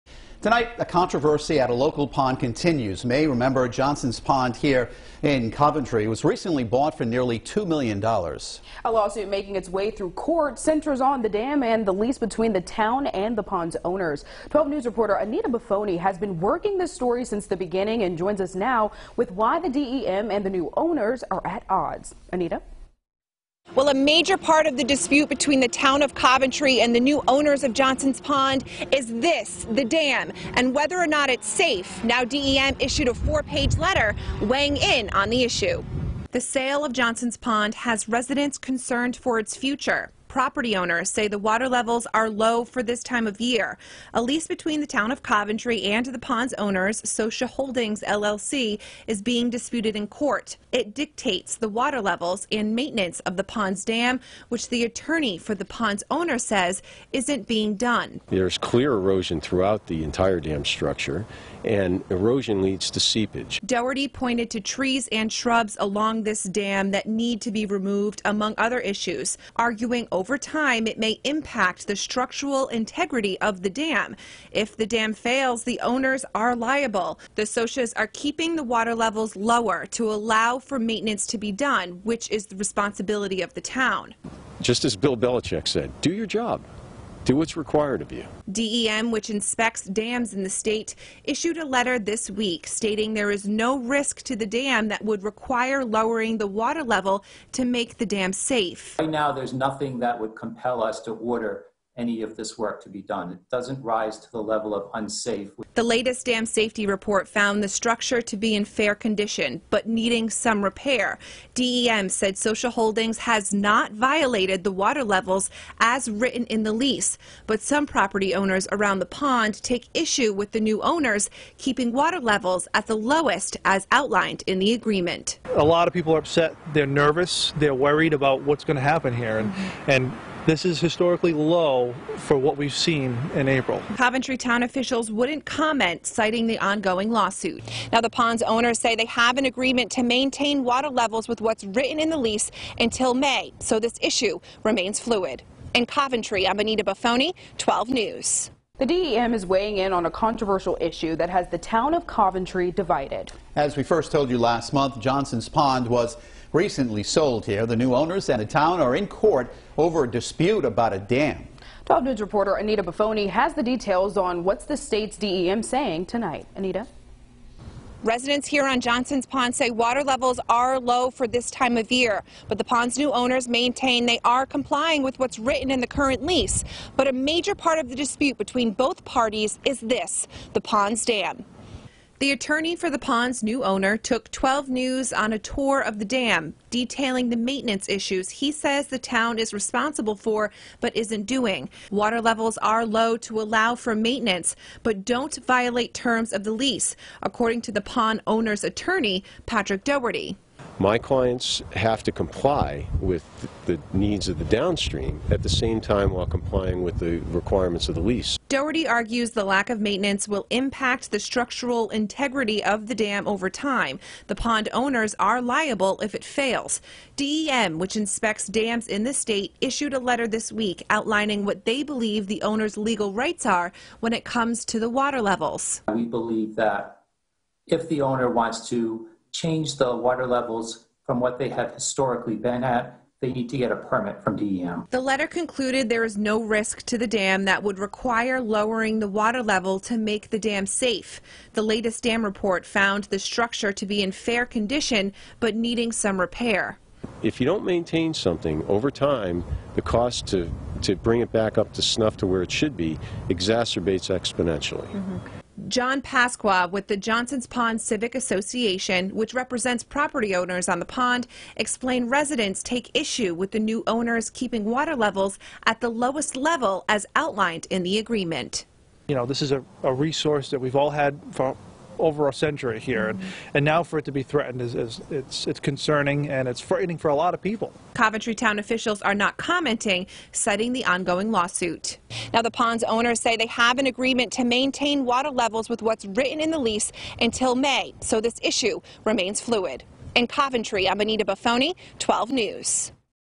RI DEM: There’s no reason for new owners of Johnson’s Pond to keep water levels low
A lease between the owners of a Coventry pond caught in the middle of a rift with the town is being disputed in court.